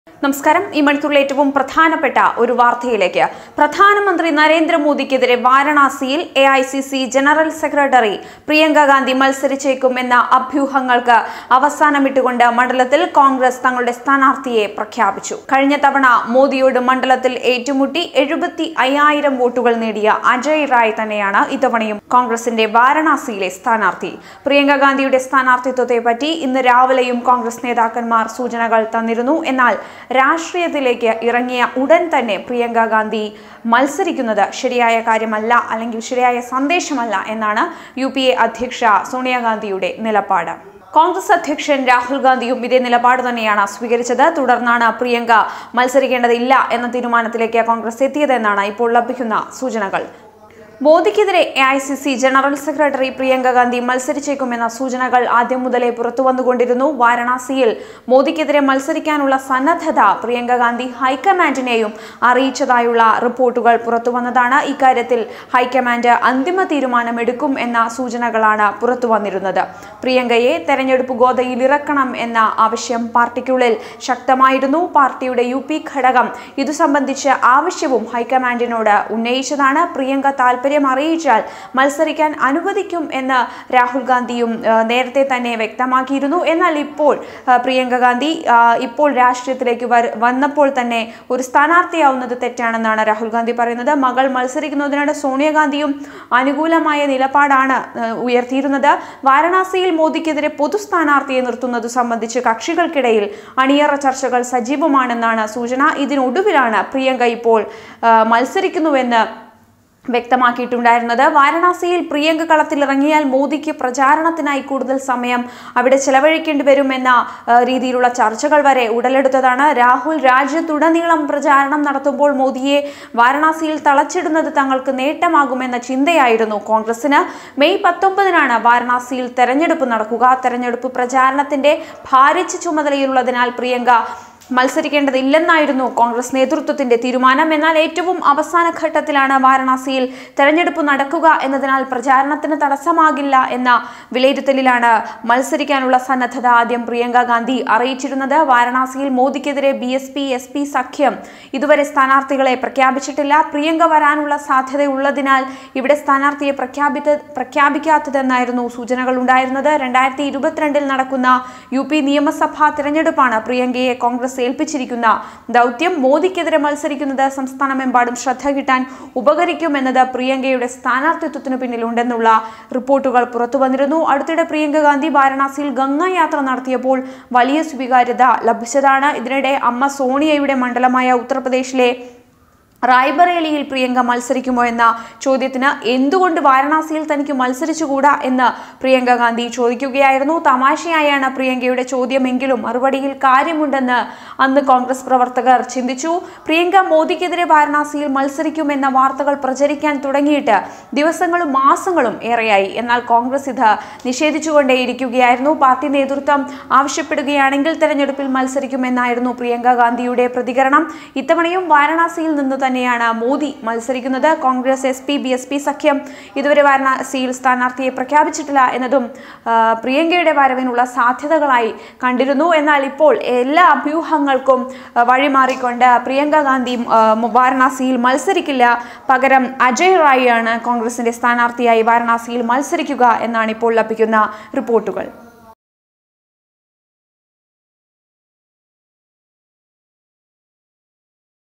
പ്രിയങ്ക വാരണാസിയില്‍ മത്സരിക്കില്ല I Priyanka Gandhi
പ്രധാനമന്ത്രി നരേന്ദ്ര മോദിക്കെതിരെ വാരണാസിയില്‍ എ.ഐ.സി.സി ജനറല്‍ സെക്രട്ടറി പ്രിയങ്കാ ഗാന്ധി മത്സരിച്ചേക്കുമെന്ന അഭ്യൂഹങ്ങള്‍ക്ക് അവസാനമിട്ട് മണ്ഡലത്തില്‍ കോണ്‍ഗ്രസ് തങ്ങളുടെ സ്ഥാനാര്‍ത്ഥിയെ പ്രഖ്യാപിച്ചു. കഴിഞ്ഞ തവണ മോദിയോട് മണ്ഡലത്തില്‍ ഏറ്റുമുട്ടി 75,000 വോട്ടുകള്‍ നേടിയ അജയ് റായ് തന്നെയാണ് ഇത്തവണയും കോണ്‍ഗ്രസിന്റെ സ്ഥാനാര്‍ത്ഥി
Priyanka Gandhi